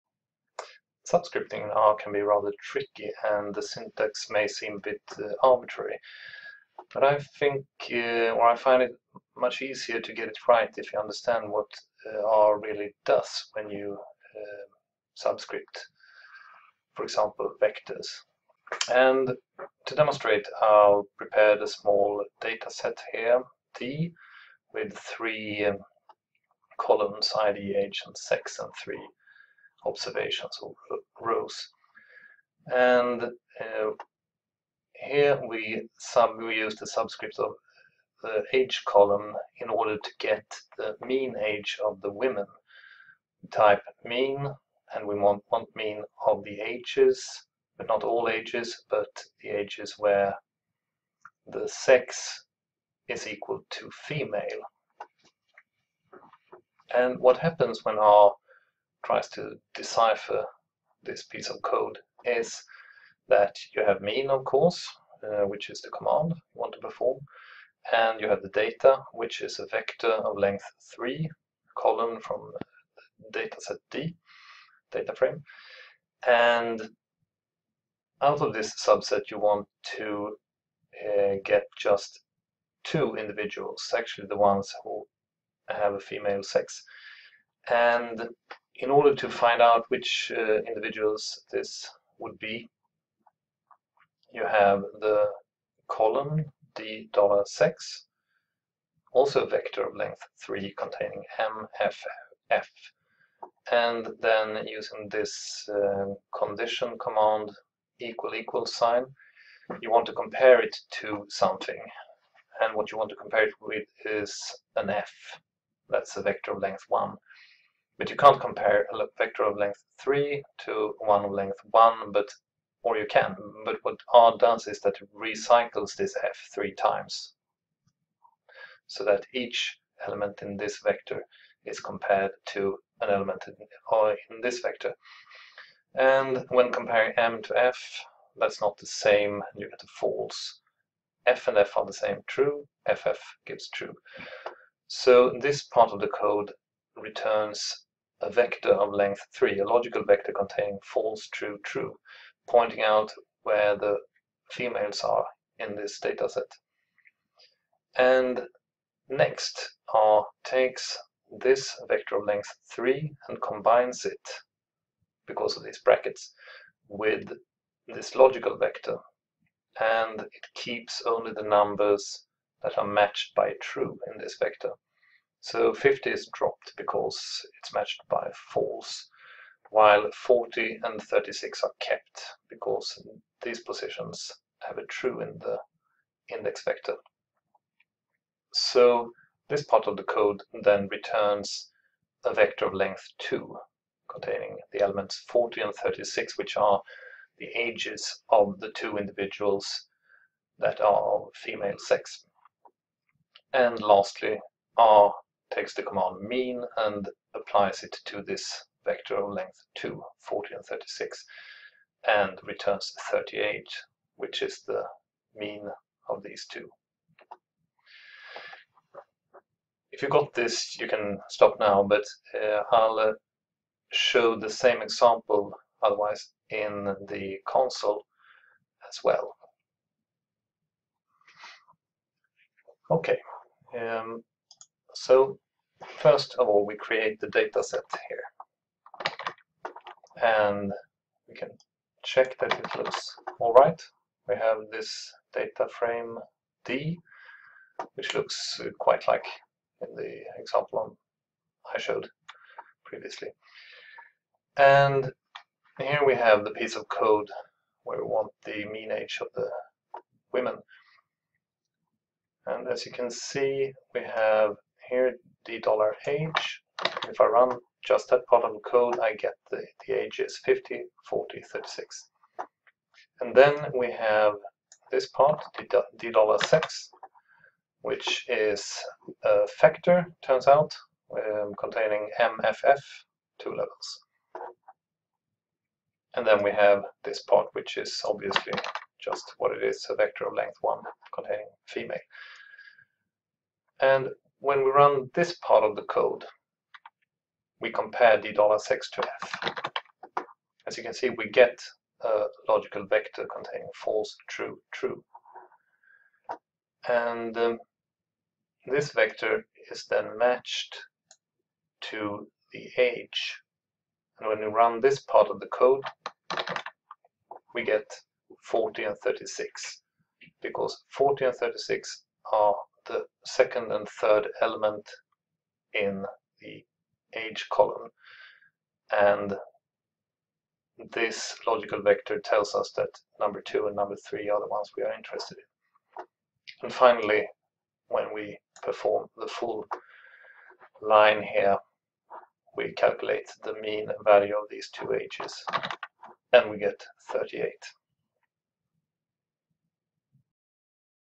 Subscripting in R explained
Making selections in R can be confusing. Here's how it really works!